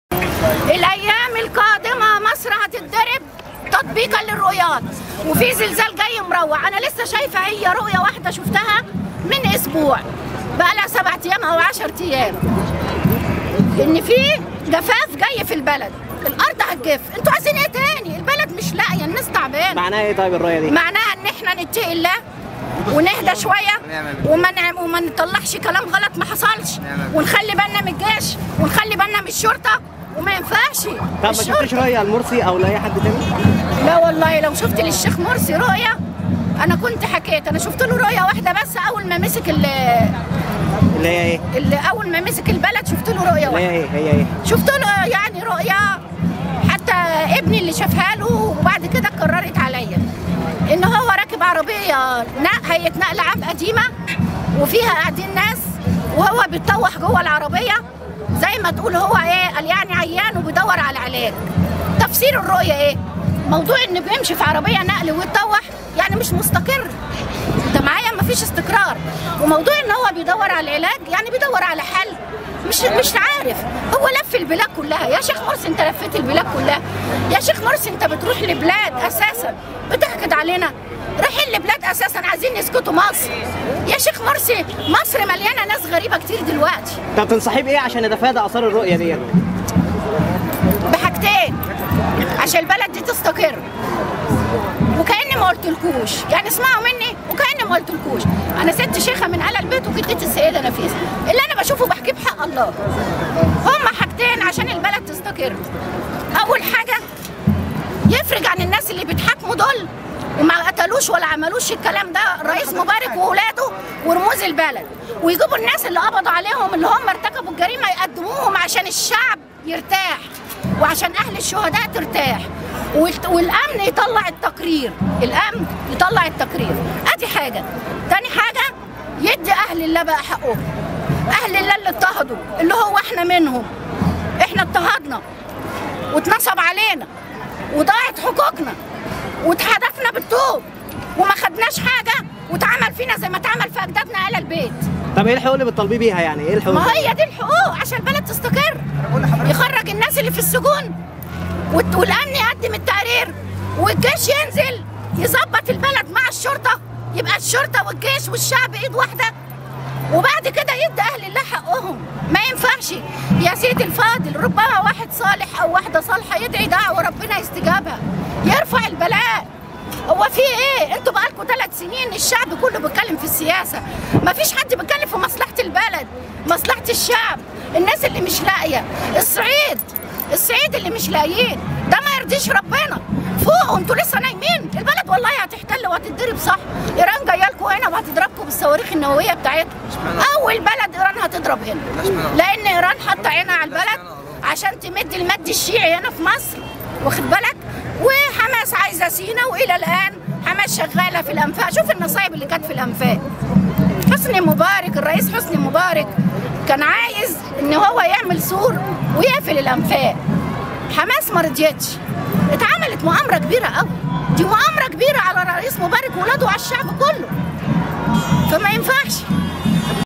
الشيخه ماجده اما اكاديميه الشرطه
الشيخه ماجده امام اكاديميه الشرطه اليوم السبت الموافق13/4/2013 تدعو الشيخ مرسى للافراج عن المسجونين